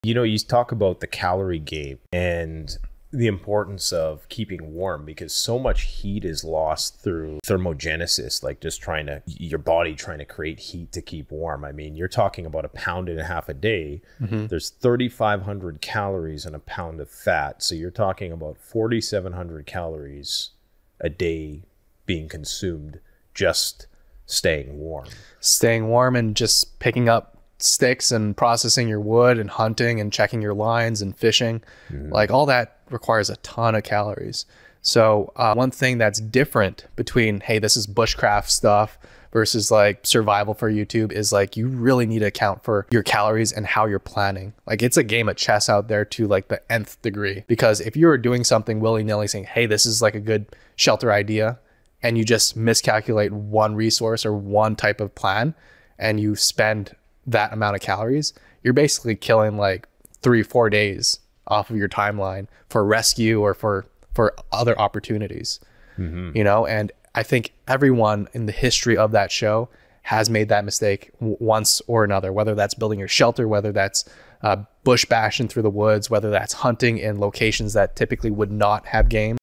Mastering Calories: Essential Survival Strategies for Bushcraft
Calorie management is crucial in survival situations. This video delves into the importance of thermogenesis and resource planning in bushcraft, ensuring you stay warm and efficient while maximizing your chances of rescue. Learn how every decision impacts your energy and survival.